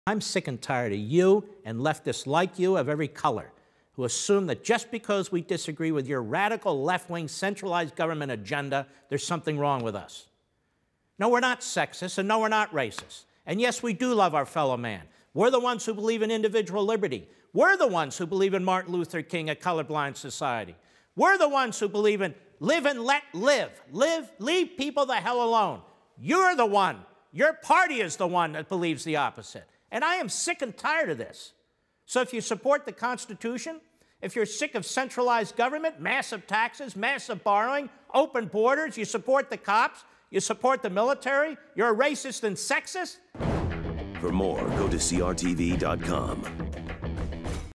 Leftists Assume the Worst
People are being called sexist and racist if they support as much as one of Trump’s ideas. Absurd!

Take a break from the mainstream media.